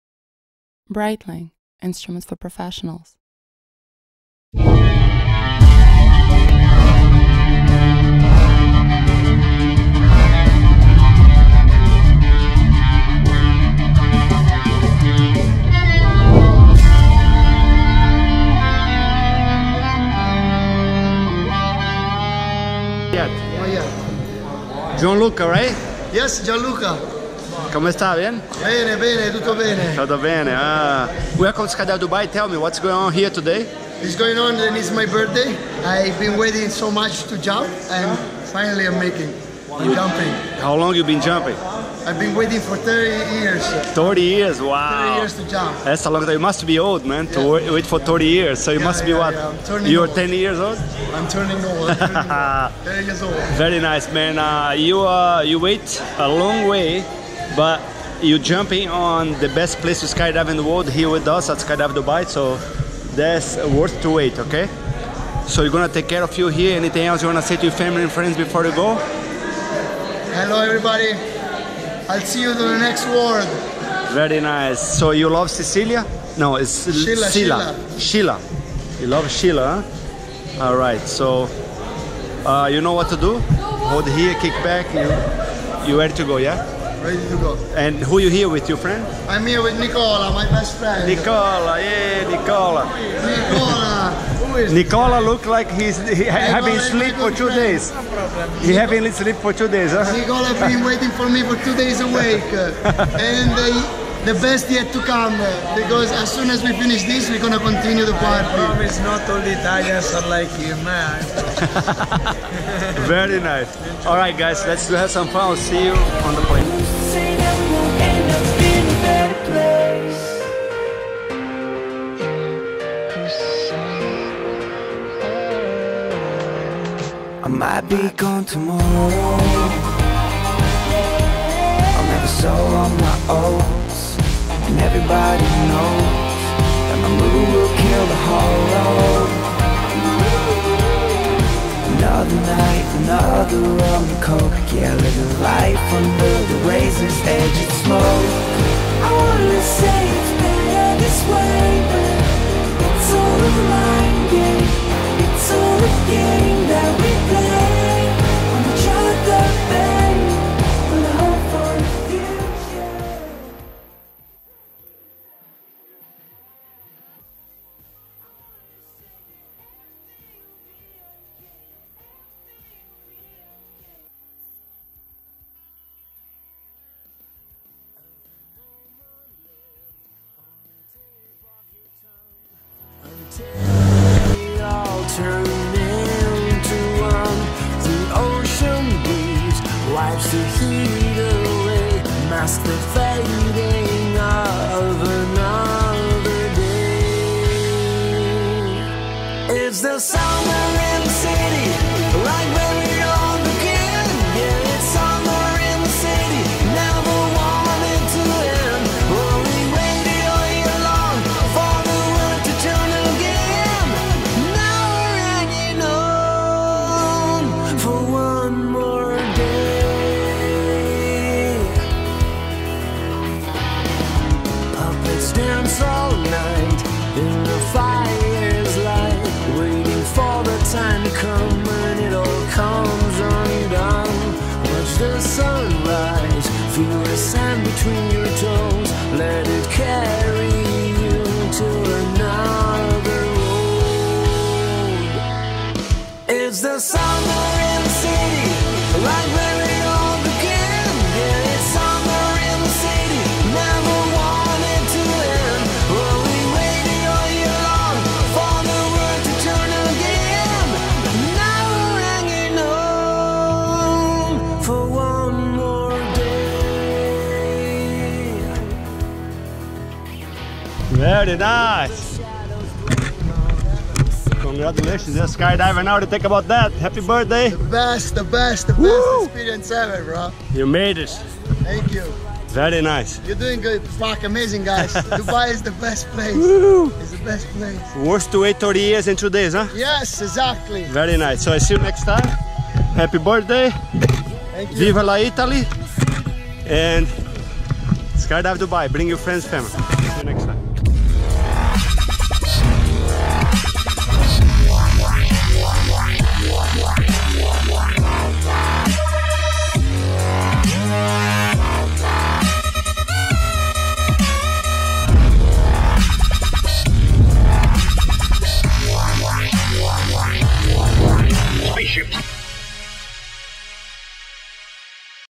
MY BIRTHDAY GIFT Skydive Dubai
Nothing better than welcome my 30th Birthday.. A fantastic one of a kind experience. After many years of dreaming it... I made it on a special Day... SkydiveDubai is the best place ever where you can jump from the sky.